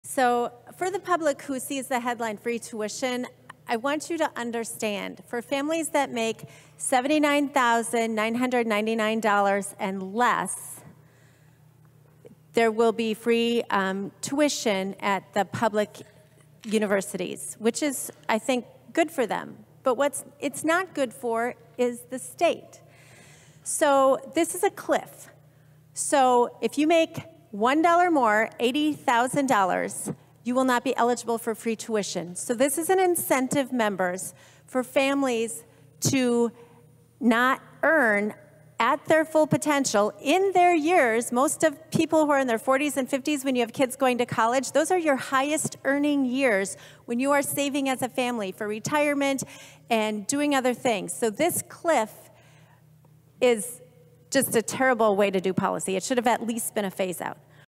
MN State Rep. Robbins (R) says giving families free college will cause them to stop earning money
MN State Rep. Kristin Robbins (R-Maple Grove) says that giving families making under $80k free college will cause parents to stop trying to earn money in order to cash in on it:

"This an incentive for families to not earn at their full potential."